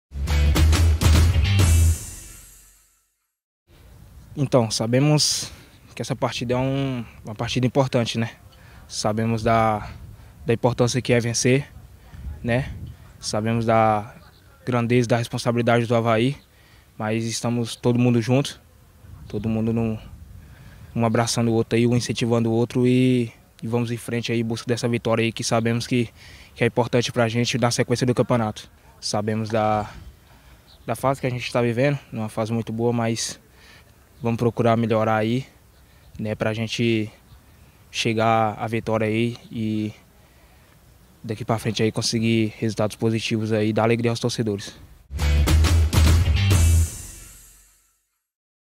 Atacante Paulo Baya fala sobre o duelo contra o Joinville
Vídeo: TV Avaí